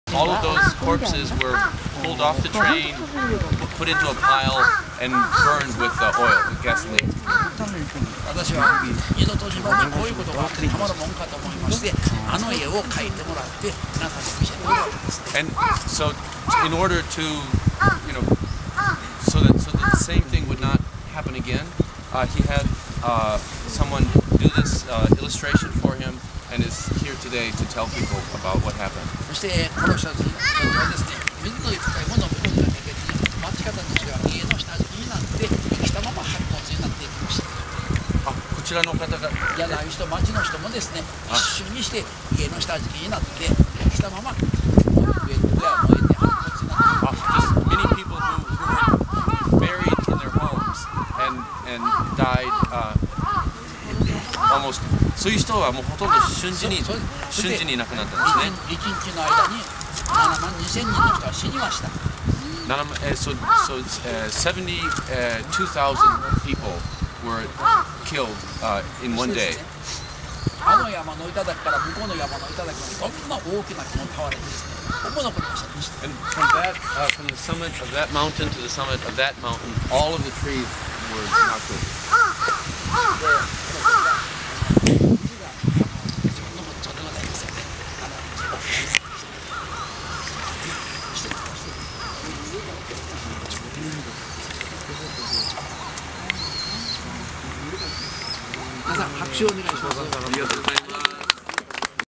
Nagasaki Atomic Bomb survivor, Mr Hayasaki
Mr Hayasaki in Nagasaki peace park. Currently 79 years old.
Mr Hayasaki survived the Nagasaki atomic bombing just 3 days after Hiroshima was hit.